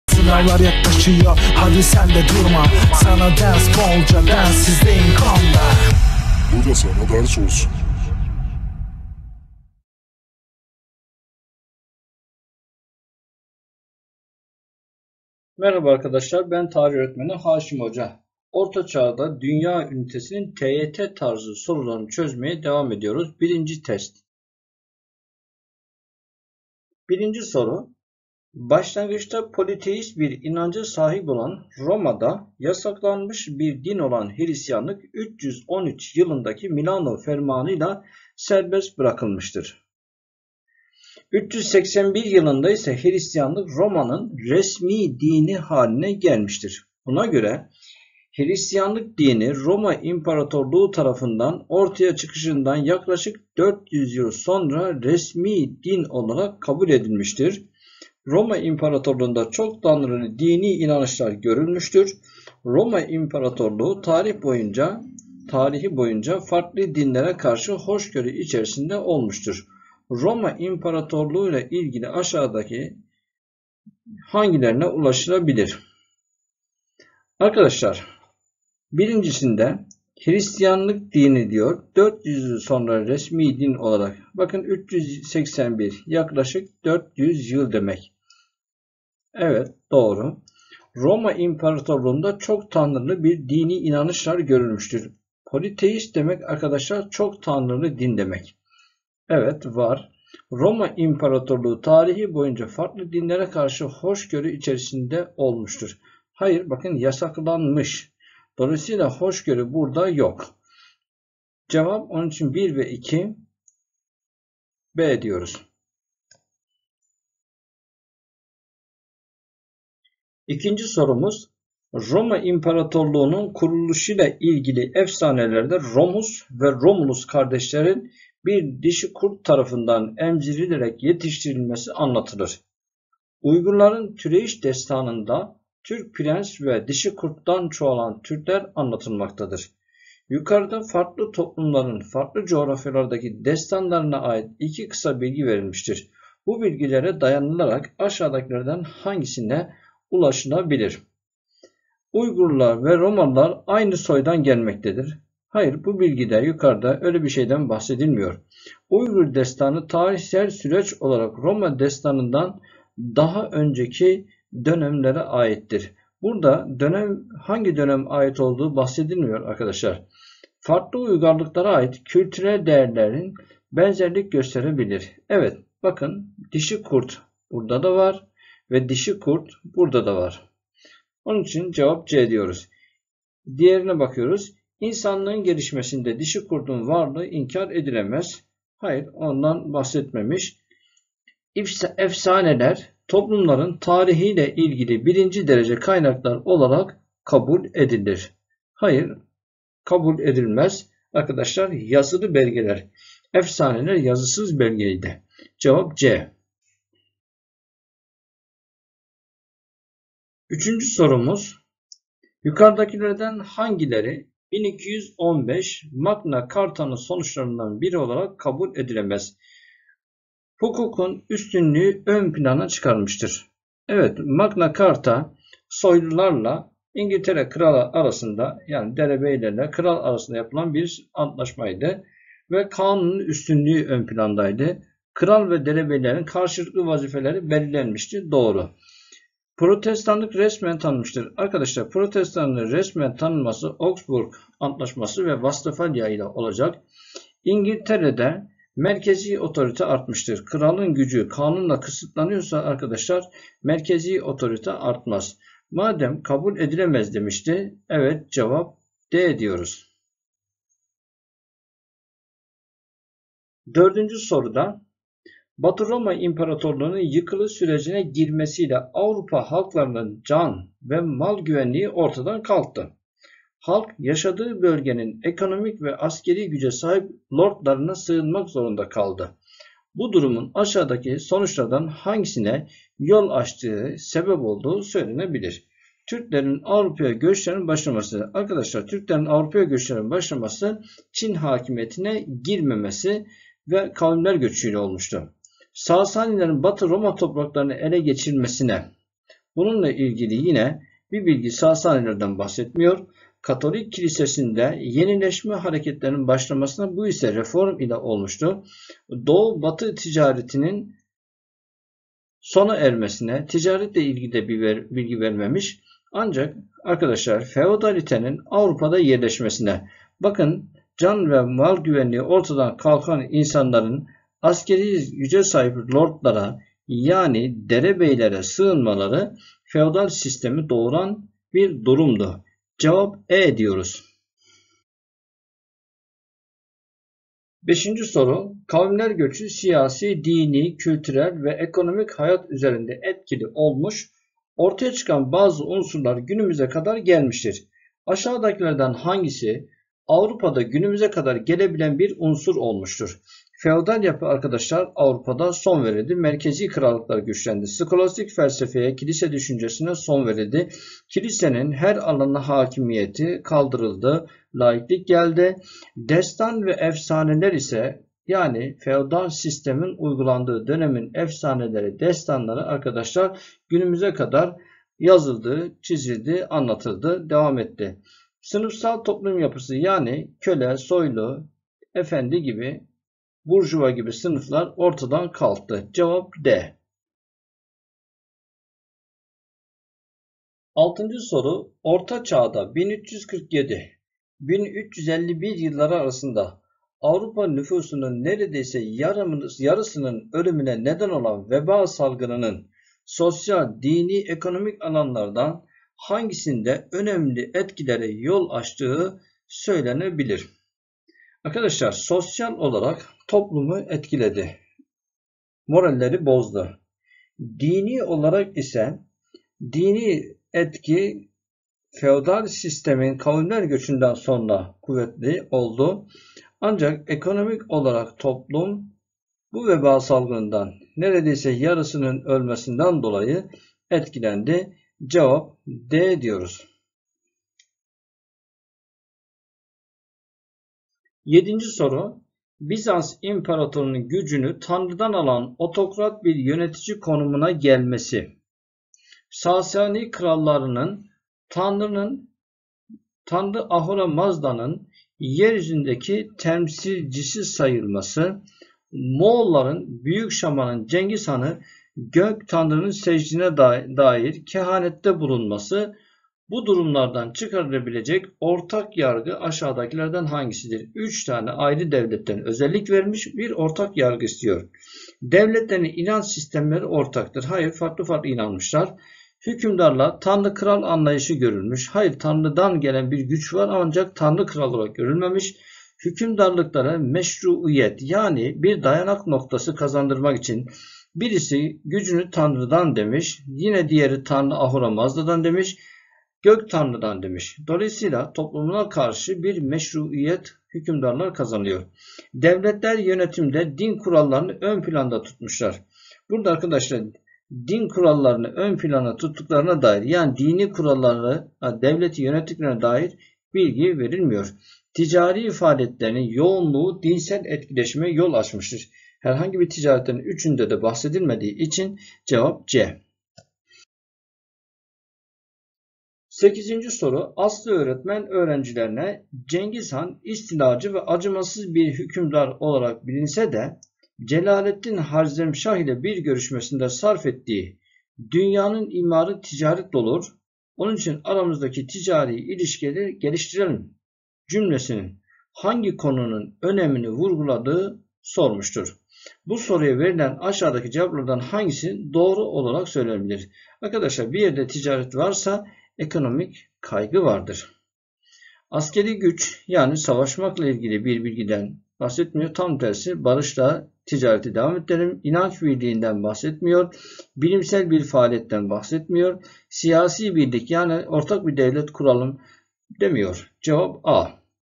ORTA ÇAĞDA DÜNYA / SORU ÇÖZÜMLERİ - 1 (TYT)
Anlatımda kullanılan sorular Delta Kültür Yayınlarının TYT Tarih Atak Soru Bankası kitabından alınmıştır. Destekleri için Delta Kültür Yayınlarına teşekkür ederiz.

Liseler için hazırladığımız bu Tarih videomuz İlk ve Orta Çağda Dünya konusunun TYT tarzı Sorularının 1. videosudur. Dersi takip ederek konuyu öğrenebilir ya da bilgilerinizi pekiştirebilirsiniz. İyi seyirler ve başarılar.